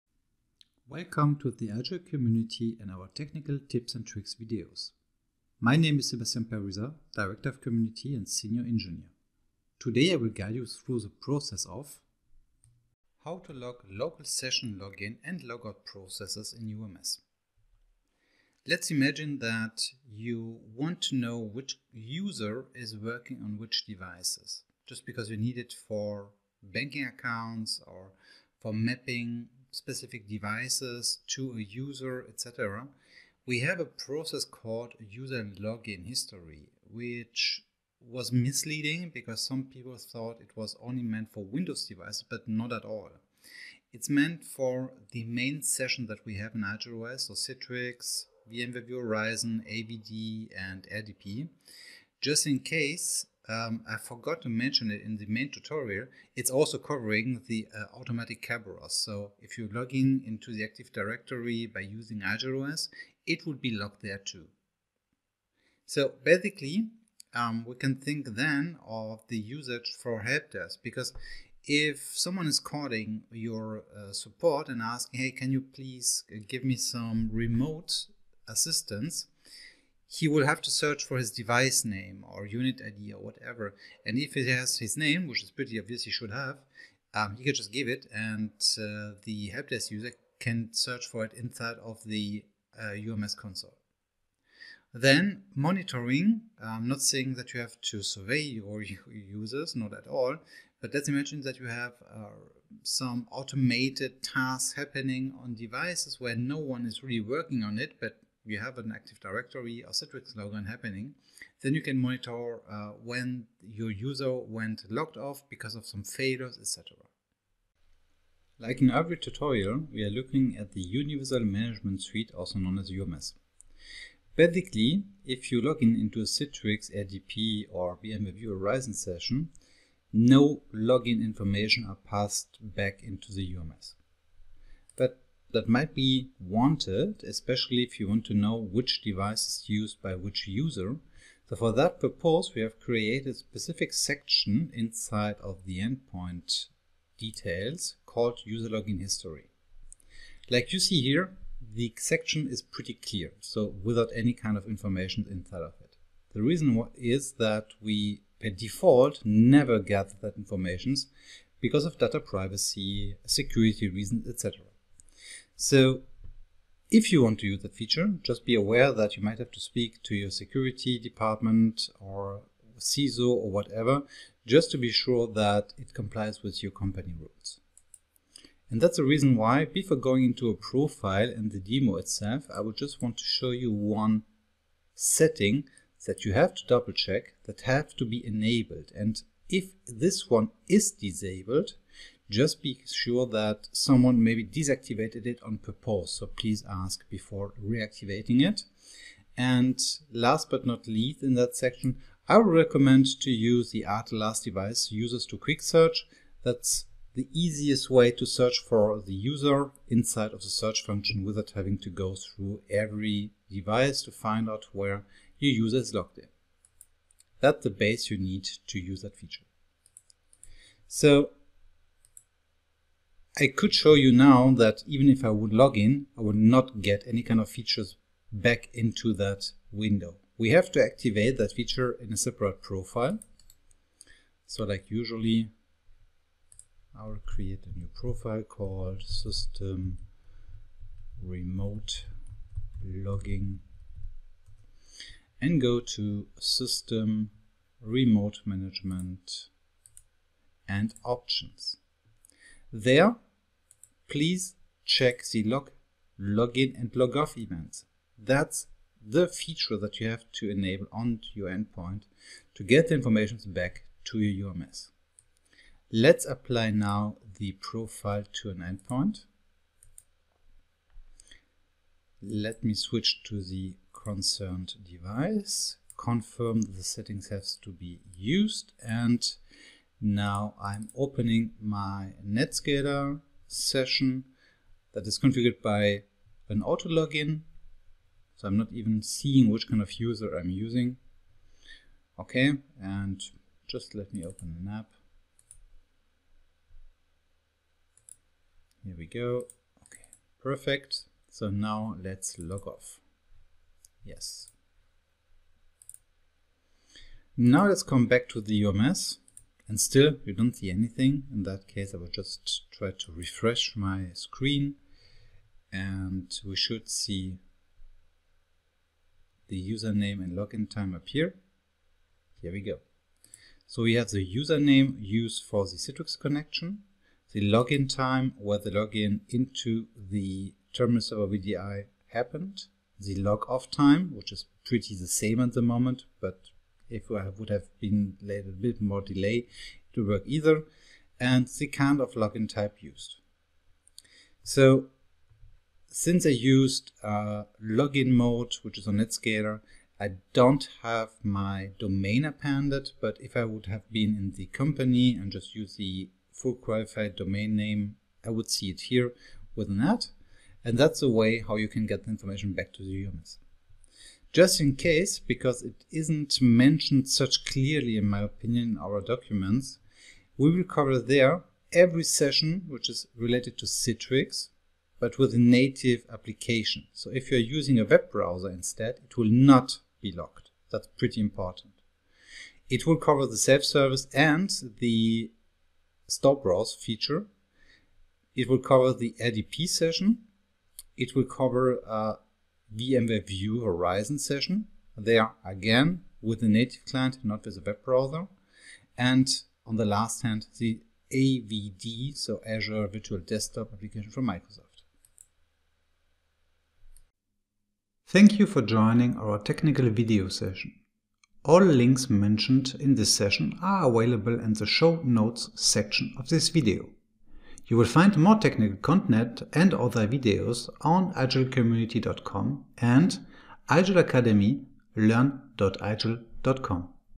How to Log Local Session Logon and Logouts in IGEL UMS - Video
Watch this IGEL Tech-Tips Tuesday video to learn how to configure the IGEL UMS to log IGEL OS local session login and logouts.

Presented by Sebastian Perusat, IGEL Community FELLOW.

In this video, Sebastien will teach you how to:

00:00 Introduction
02:00 Compatible Session types
02:25 Where to find User Login History
03:00 Configure UMS
03:50 Create a profile for it
06:10 Limitations

Not an IGEL Community Member?